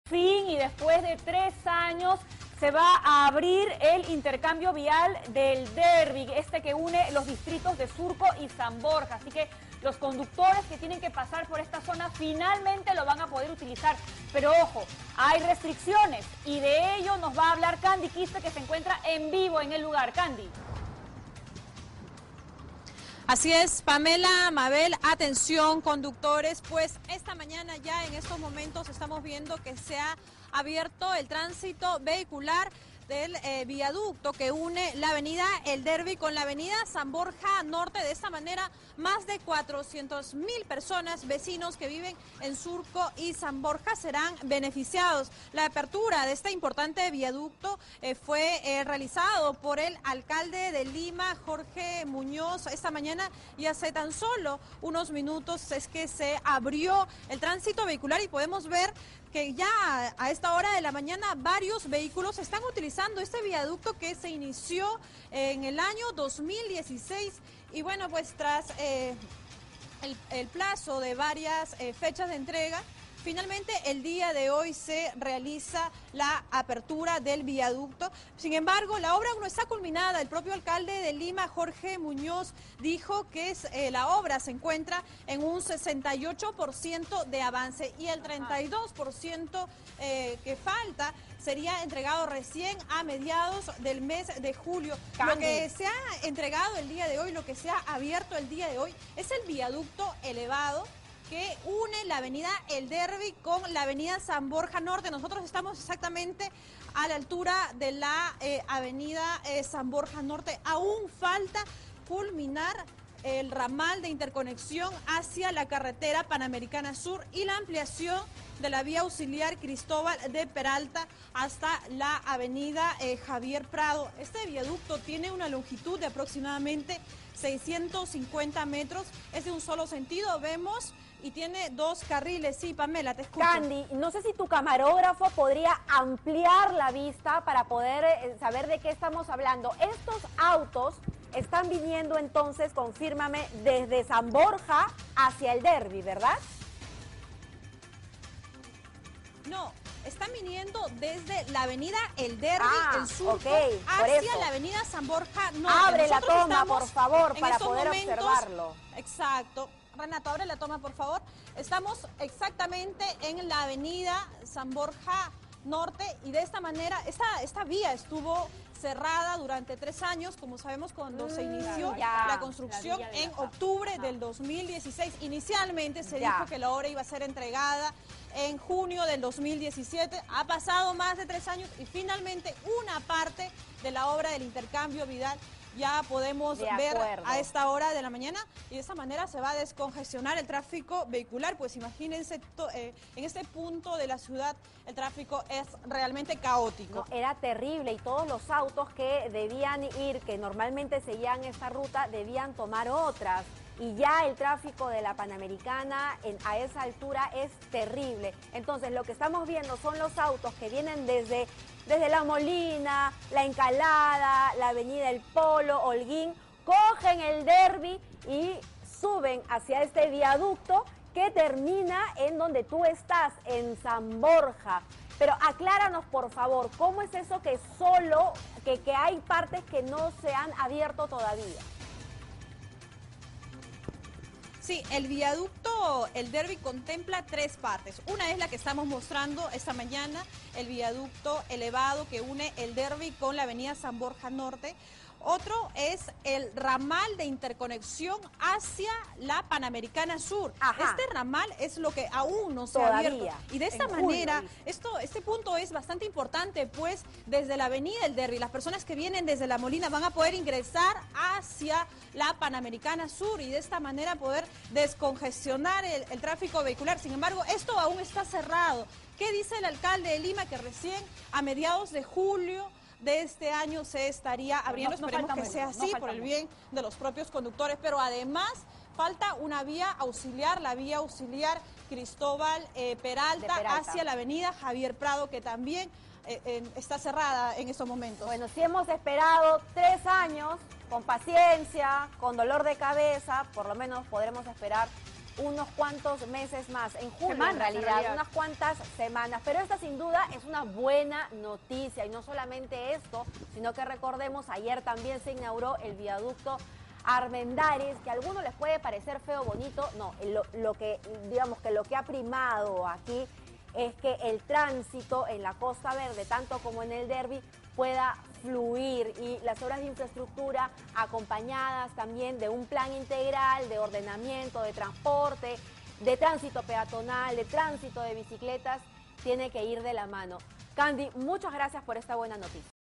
Abren viaducto del intercambio vial El Derby que unirá Surco y San Borja
El alcalde de Lima, Jorge Muñoz, abrió este jueves al tránsito vehicular en una parte del intercambio vial El Derby. Esta importan obra beneficiará a más de 400 mil personas en los distritos de Surco y San Borja.

Un equipo de Buenos Días Perú llegó hasta el lugar y corroboró que los vehículos ya circulan por esta importante vía. El viaducto tiene una longitud de aproximadamente 650 metros de un solo sentido y con dos carriles.

Sin embargo, la obra aún no está culminada y el propio burgomaestre mencionó que la obra está avanzada al 68%.La parte del intercambio entregada hoy es el viaducto elevado que une las avenidas El Derby con San Borja Norte.

Aún falta culminar el ramal de interconexión hacia la concurrida carretera Panamericana Sur. Además, de la ampliación de la vía auxiliar “Cristóbal de Peralta” que interconecta hasta la avenida Javier Prado.

Este 32% que aún falta sería entregado a mediados del mes de julio. Cabe resaltar, que Rutas de Lima inició la construcción en marzo del 2016 pero no se pudo inaugurar porque se presentaron retrasos.